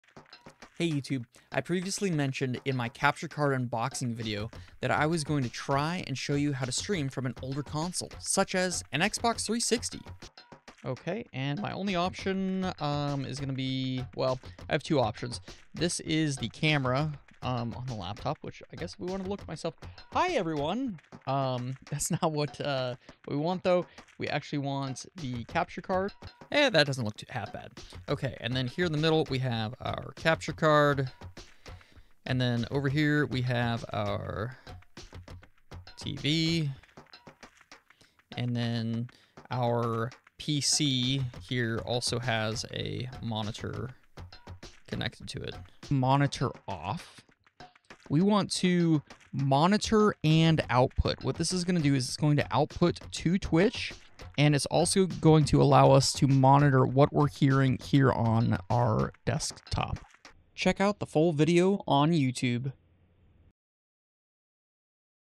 Preview: How to live stream from an older console using Linux or Windows
Check out the full video on my YouTube Channel where I walk you through how to stream to Kick, Twitch, or YouTube using either Windows or Linux. This Tutorial Should work with any gaming console that uses an HDMI port.

You can find me LIVE on Twitch most Friday and Saturday Evenings with an occasional YouTube live stream every once in a while.